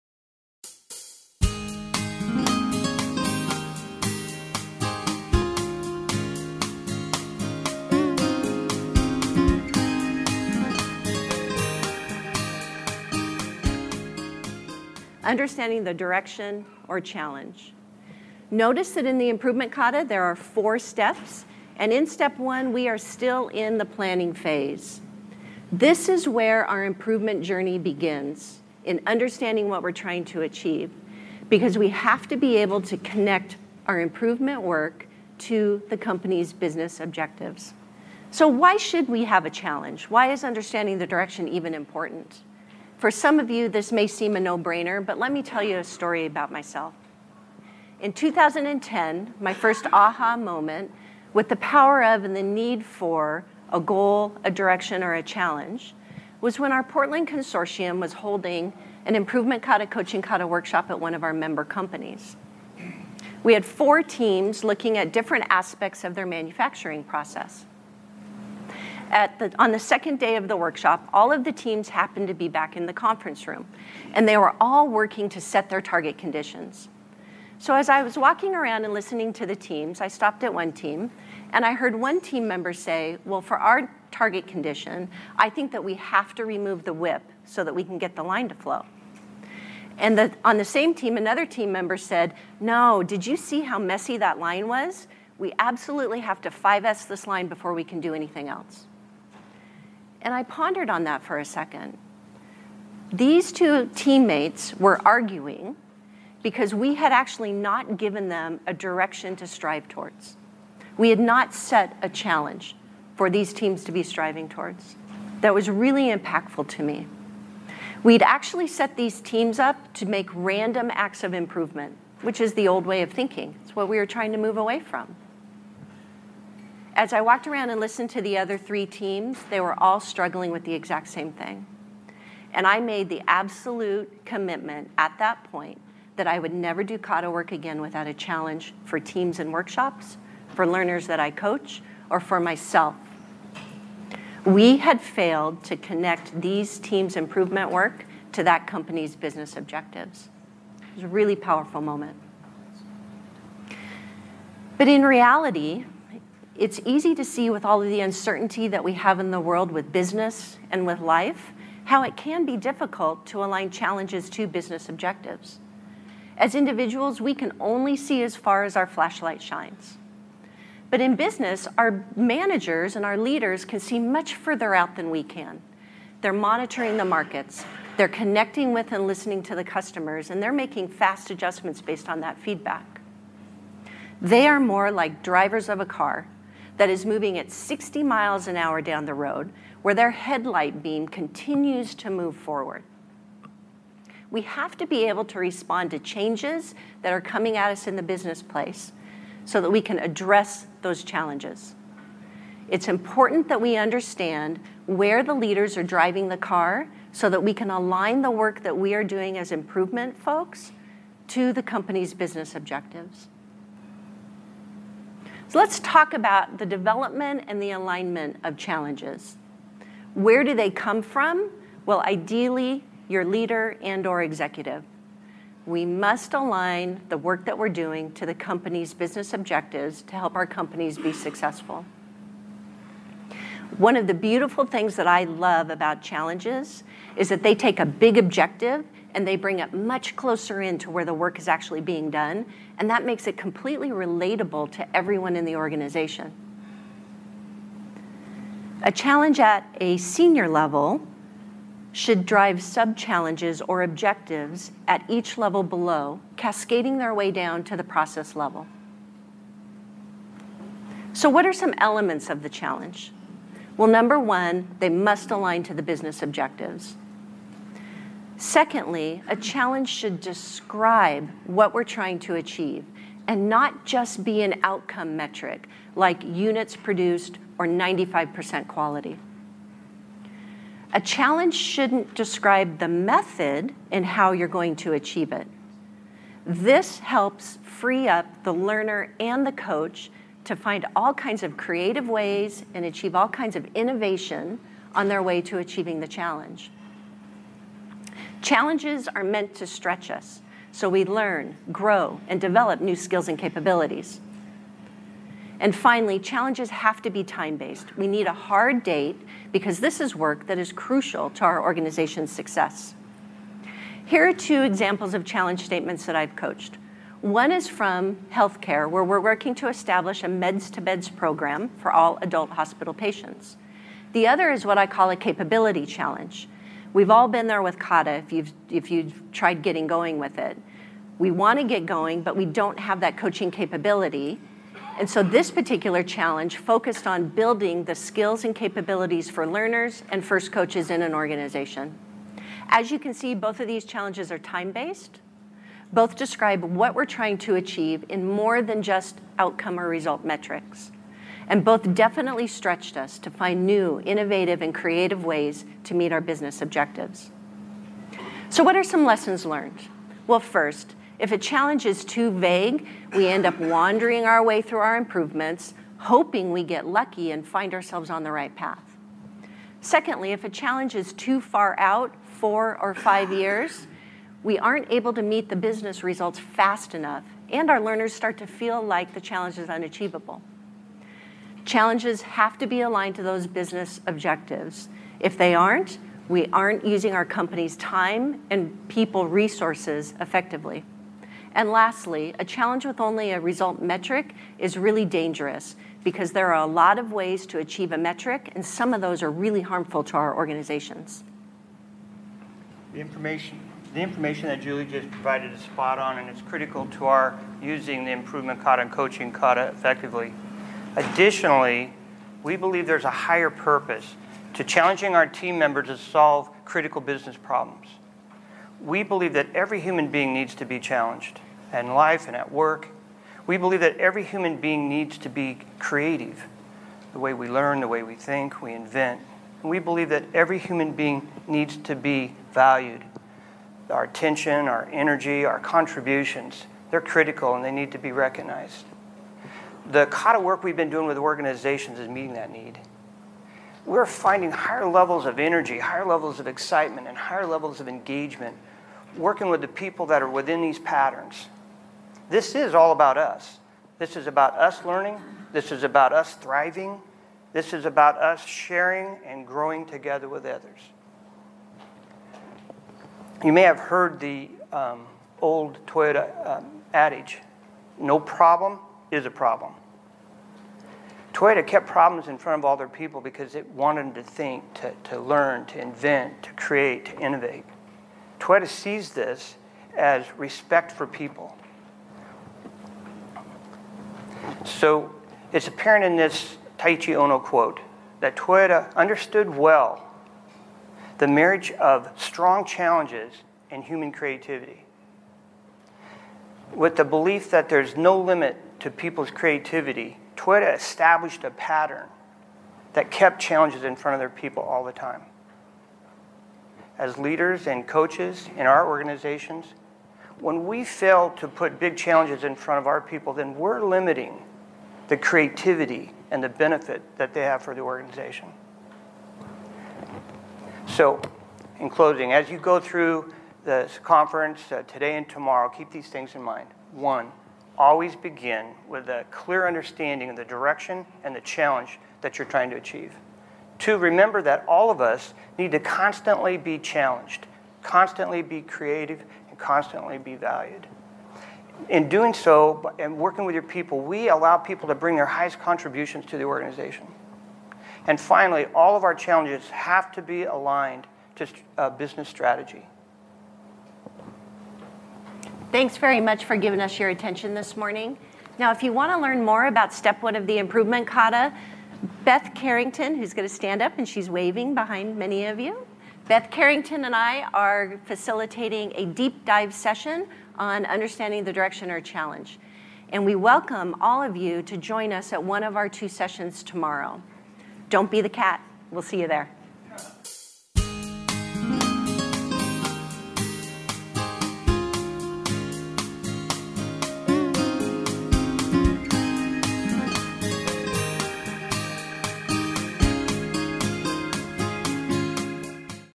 2018 Toyota Kata Summit - Keynote Step 1
Presented by Julie & Scott Simmons at KataCon4. Join us at KataCon5, February 18-19, 2019, Savannah, GA.